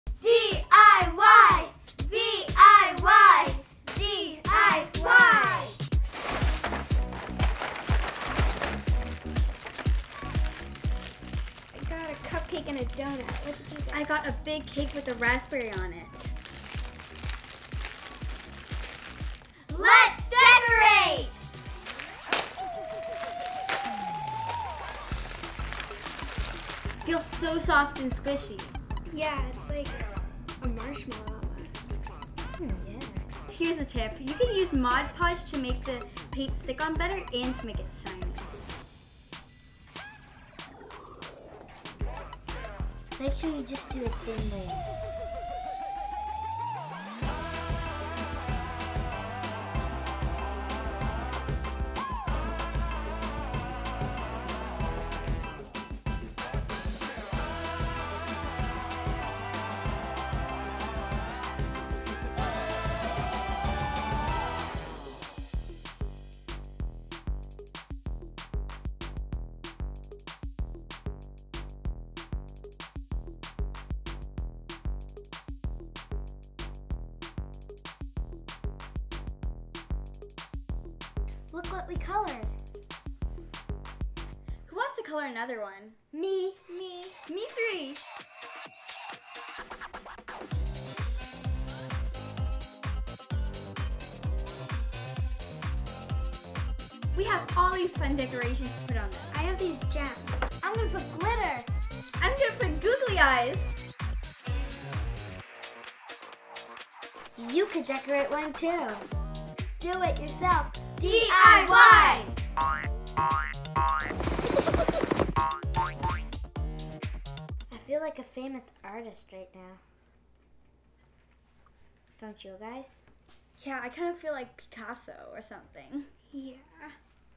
Best DIY Squishies Paint Party! Ep. 15
We had a DIY Squishies Painting Party this week! It was SO MUCH FUN! Have you tried decorating your own Soft'n Slo Squishies™ yet?!

[Available at Michaels]